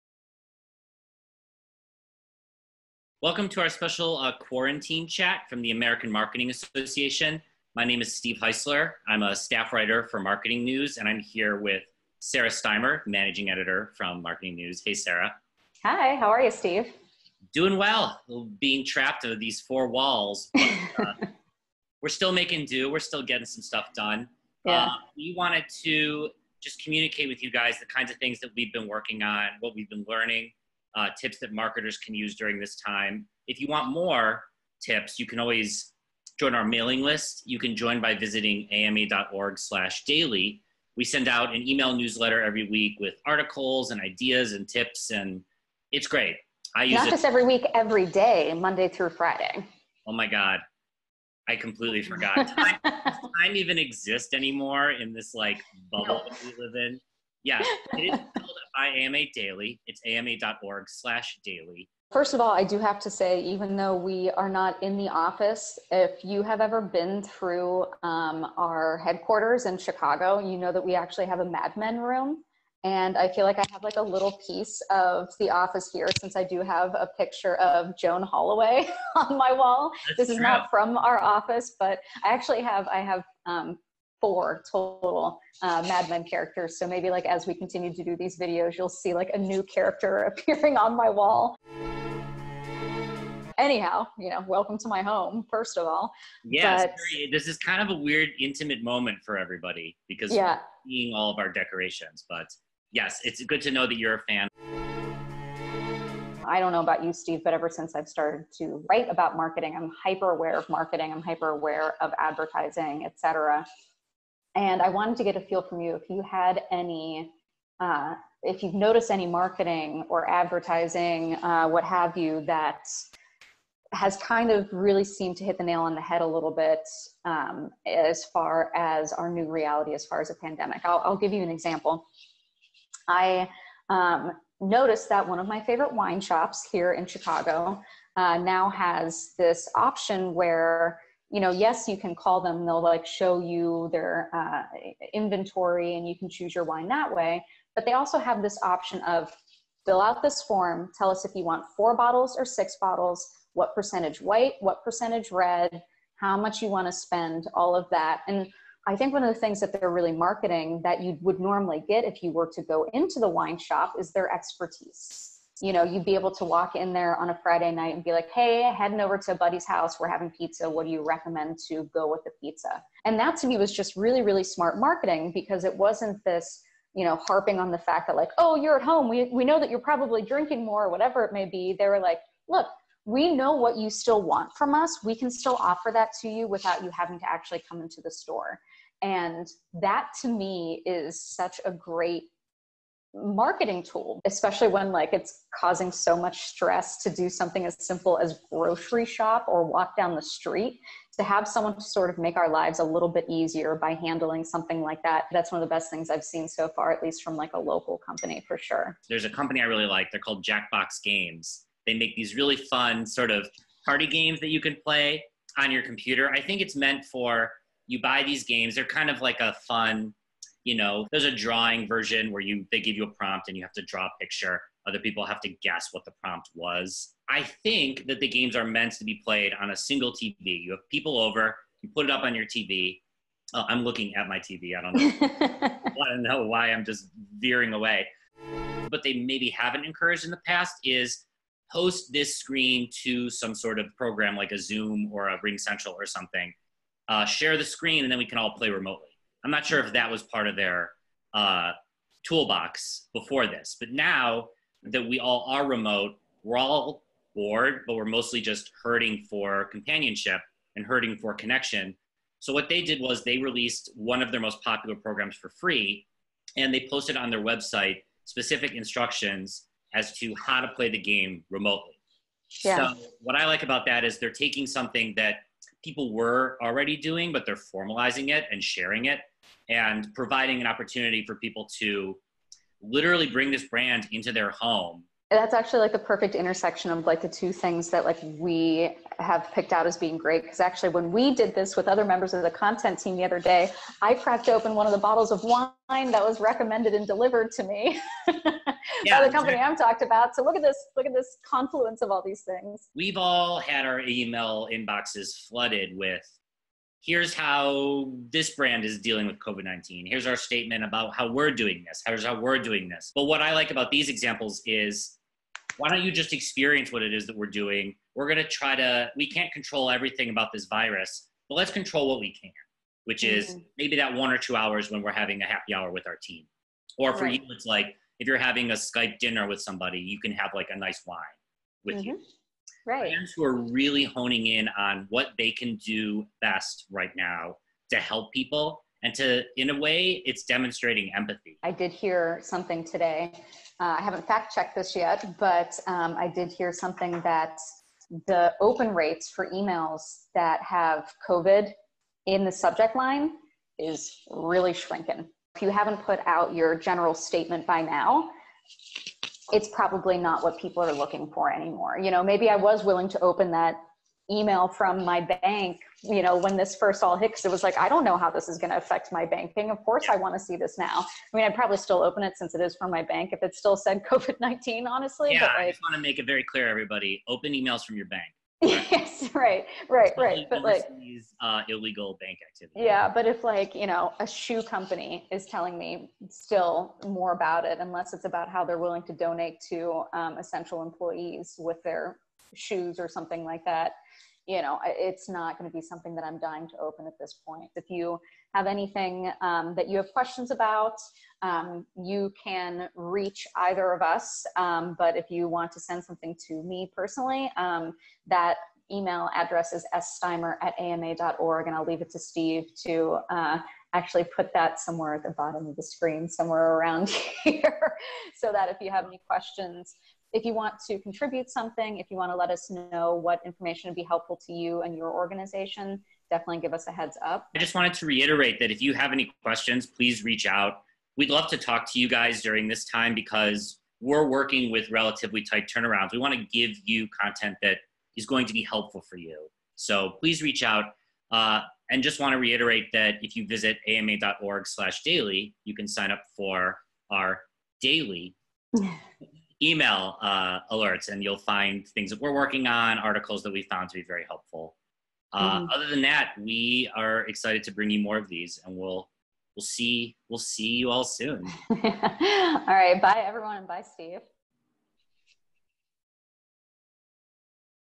Be At Home With AMA Ep. 1
Direct from the American Marketing Association's home offices, Marketing News' Steve Heisler and Sarah Steimer share stories of how brands are bringing customer experiences directly into their homes during quarantine. Look for new episodes of Be At Home With AMA every Monday and Thursday!